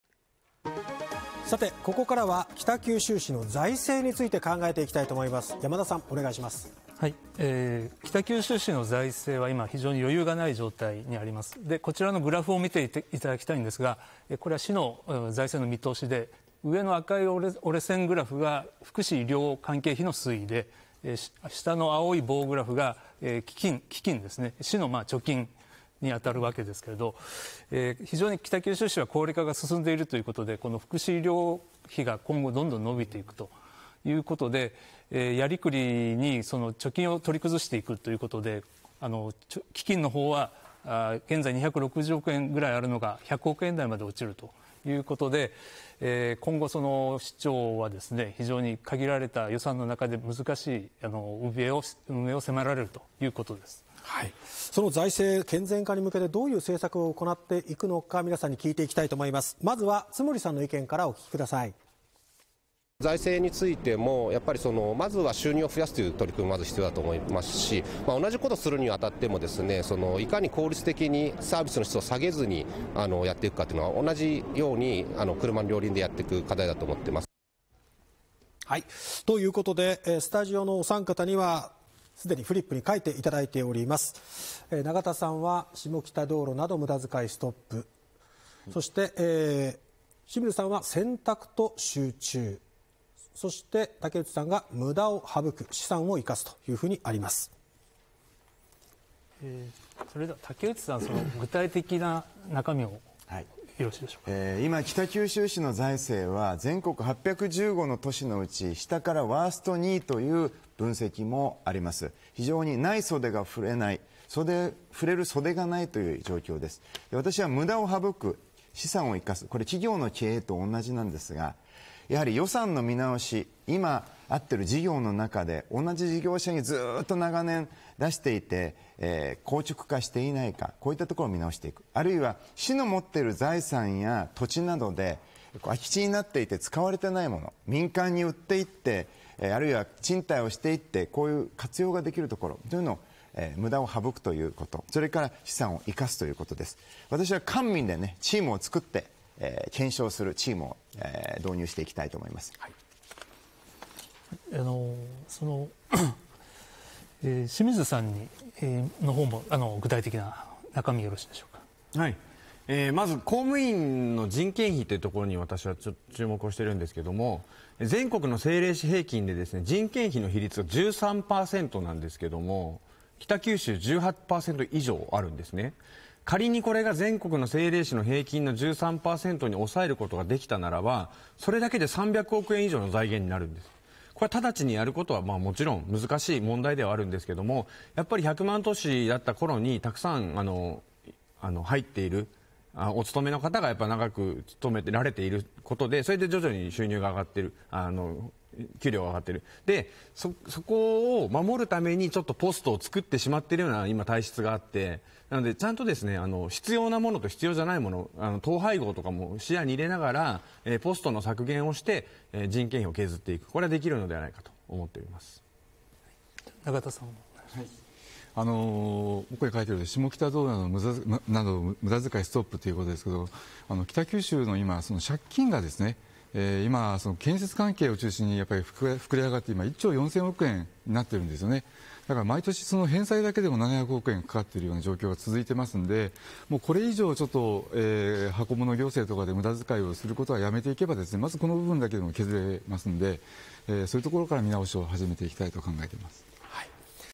もう余裕はない？北九州市から減り続ける“財政調整基金”市長選の候補者が考える財政健全化の道
かつて九州最大の都市だった福岡県北九州市。製鉄業の隆盛により経済は発展し、１００万都市へと成長した。しかし、鉄冷えと呼ばれる製鉄業の衰退や企業の移転により、今では人口は１００万人を大きく割り込んでいる。政令市の中で高齢化が最も進み、増え続ける福祉・医療関係費により財政は厳しい状況。そんな都市に２０２３年２月、１６年ぶりに新しいリーダーが誕生する。

候補者がＲＫＢのスタジオに集まった「討論会」。このパートでは、候補者が福祉と財政をどのように建て直していくか議論する。